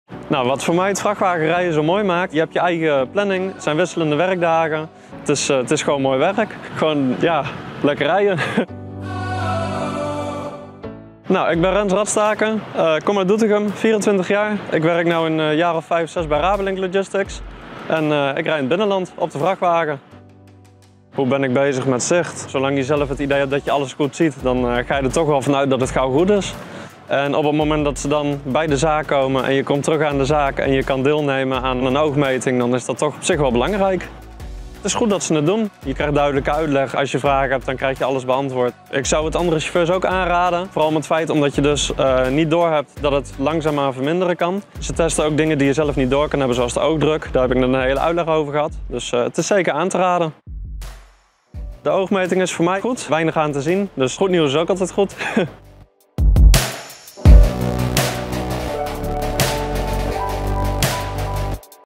Goed Voorzien: Eyewish en TVM op bezoek bij Rabelink Transport
Goed zicht in het verkeer is belangrijk. Om situaties in te schatten en in een fractie van een seconde adequaat te reageren. TVM kijkt hierin graag mee. Voor beter zicht op risico’s, minder ongelukken en meer veiligheid op de weg. Goed voorzien noemt TVM dat. Hier ziet u een impressie over medewerkers van Rabelink Transport die hun ogen konden laten testen in een bus van Eye Wish Opticiens. Wilt u persoonlijk preventieadvies of een oogmeting op locatie? Lees meer op